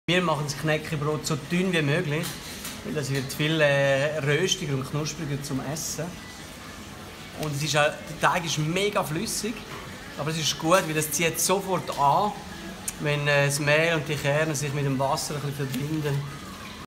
Knäckebrot mit Jann Hoffmann
Jann Hoffmann, Küchenchef vom Restaurant Café Boy in Zürich, zeigt, wie man Knäckebrot zubereitet.